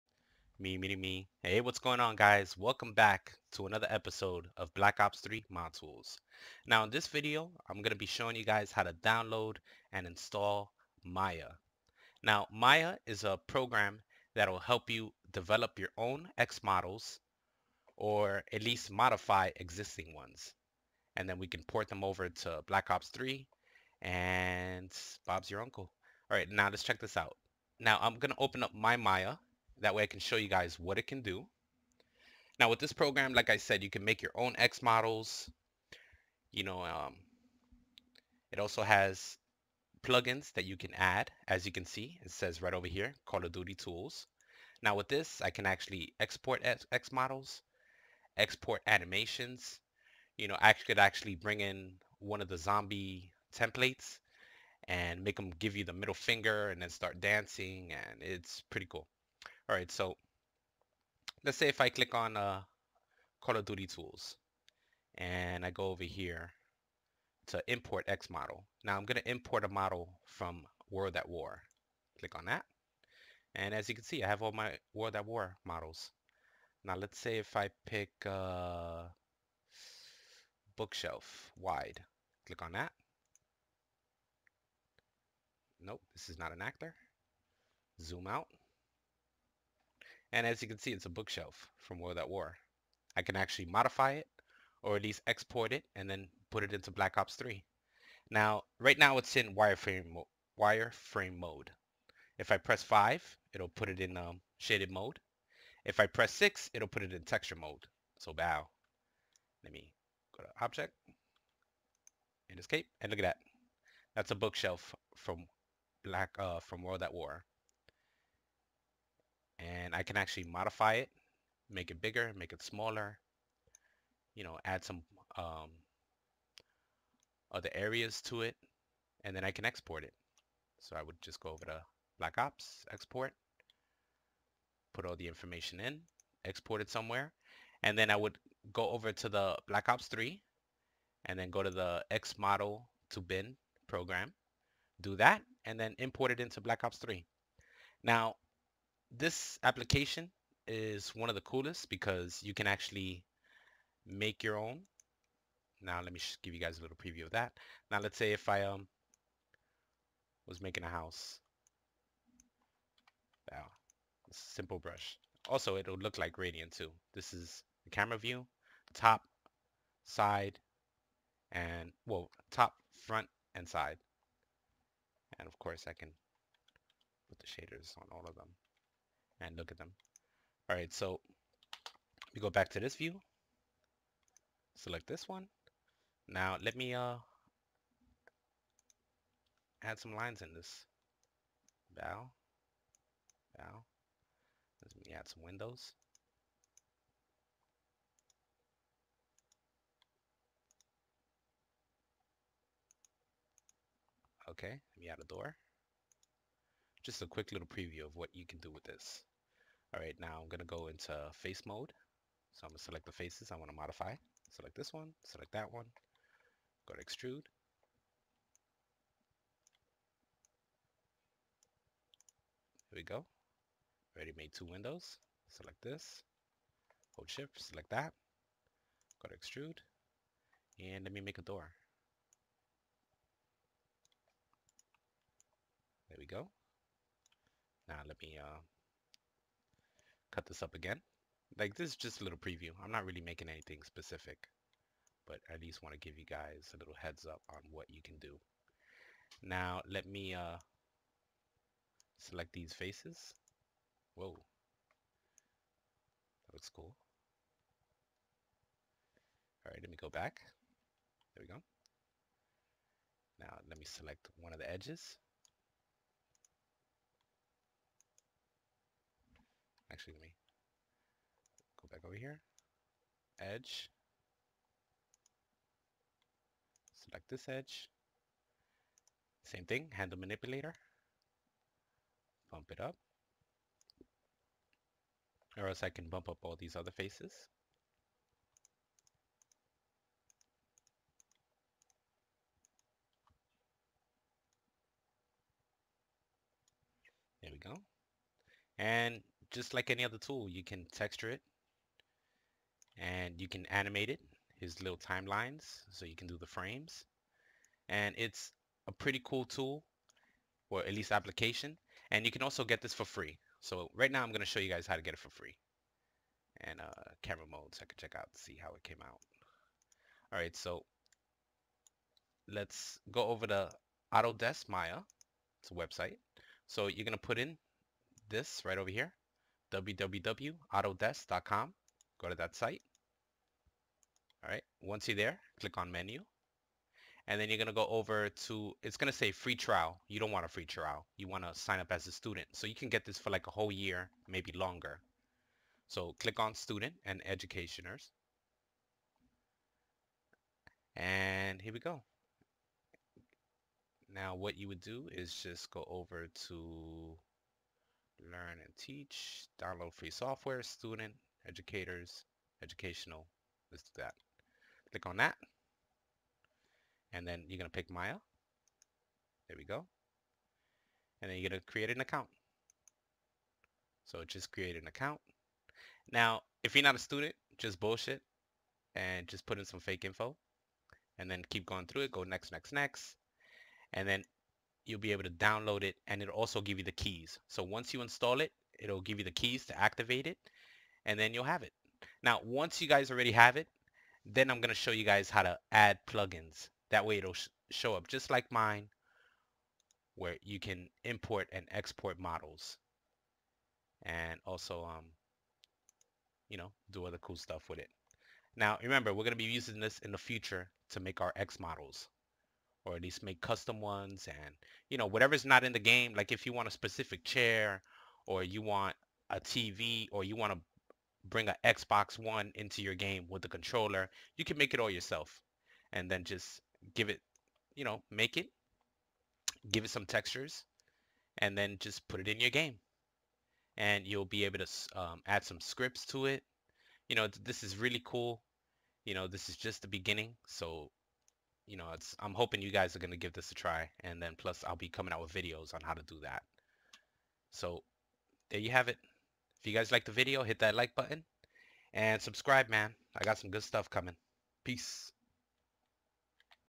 Black Ops 3 Custom Models Preview and Download Maya
video preview of what maya can do and how it works with black ops 3.
get any version of maya you want. i  have 2014 and 2015.